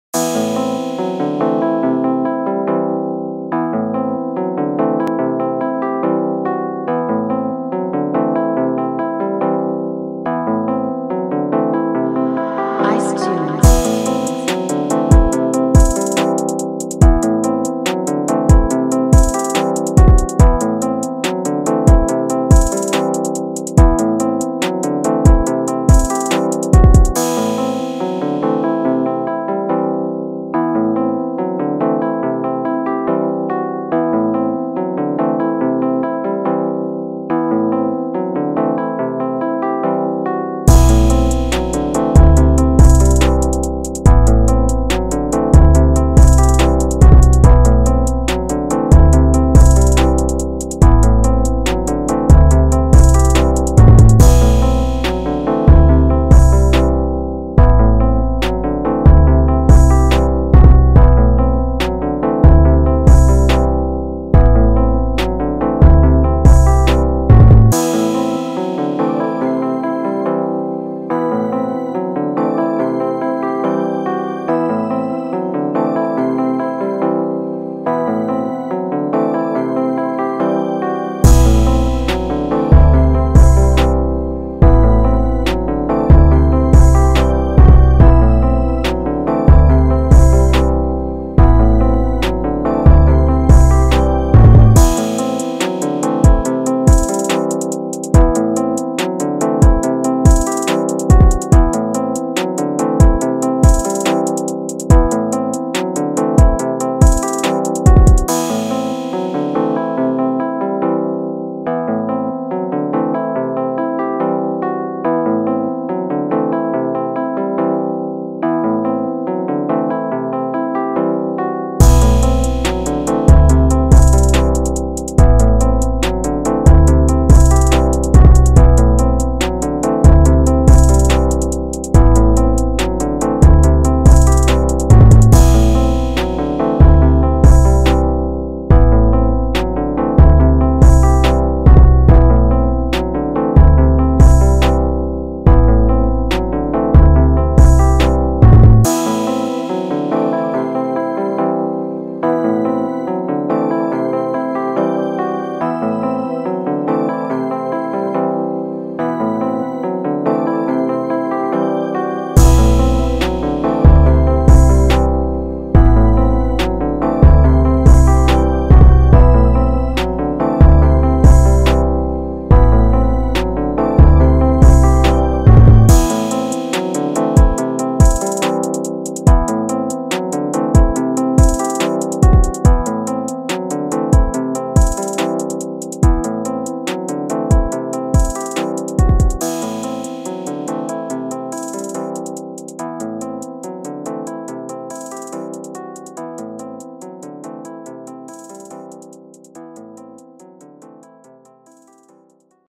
Who Deserves This Instrumental [Part 29] Moneybagg Yo, Dave East, Lotto Savage Type Beat 2020
This is a series of Instrumentals that are similar to the one used by big artists, you are required to choose who can kills the beat [ Between 3 mentioned artists ] and comment his/ her name or number in the comment section.
If you would like to purchase the beat, you can request to buy it as long as it wasn't purchased by anyone.

Trap Beats, Hip Hop beats, Beats for sale, Rap Music, Instrumentals, Rap